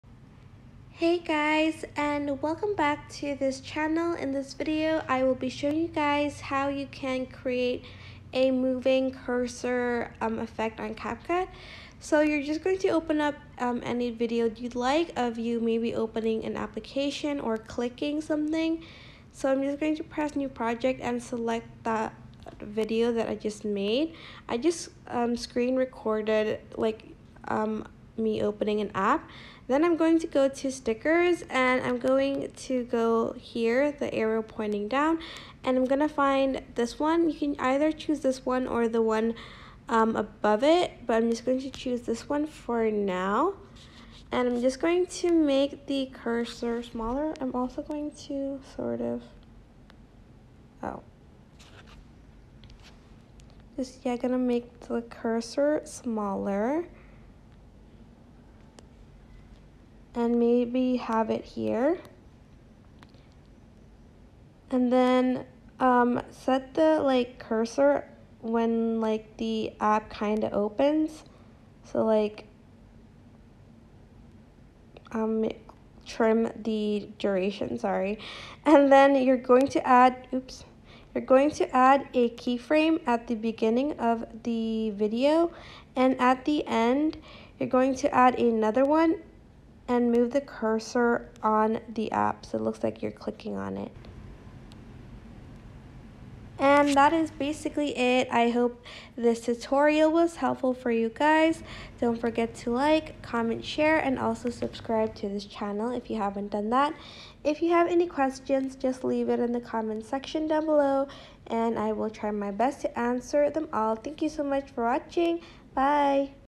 Moving Cursor / Mouse Edit Tutorial On CapCut
Create this moving cursor edit on CapCut by watching this tutorial.